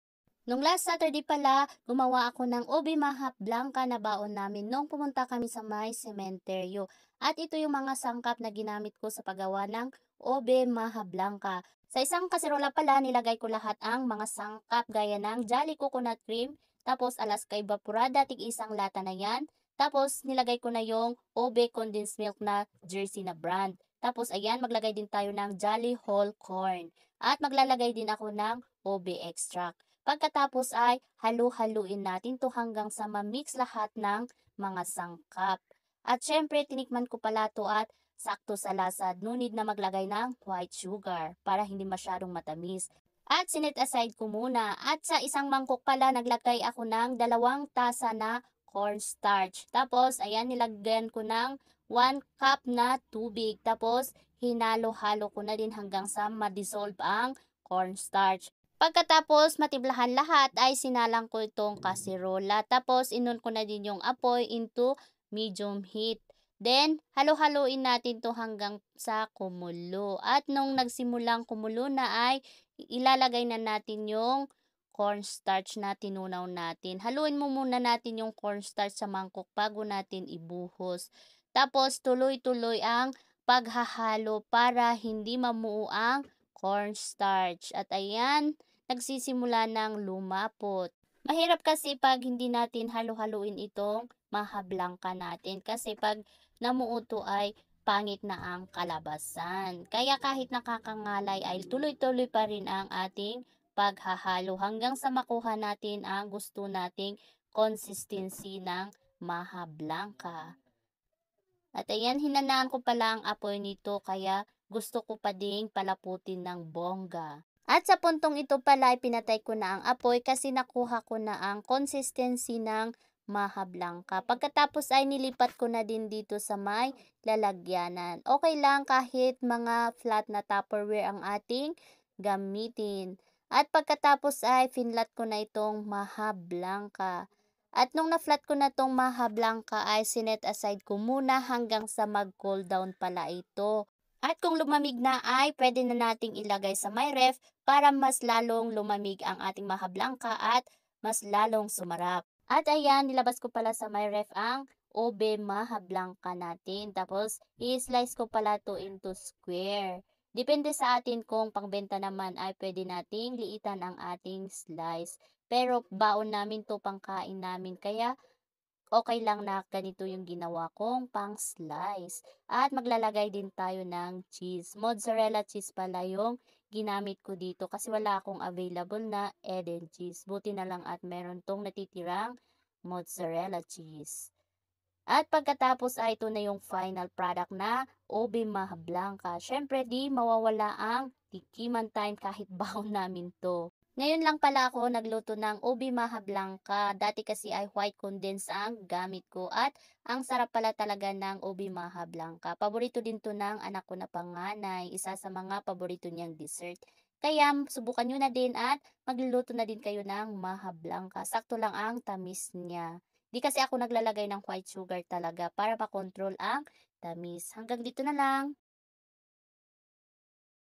Ube Maja Blanca
Super yummy ng ube maja blanca ang bango din kasang ube kaya gawin mo na din.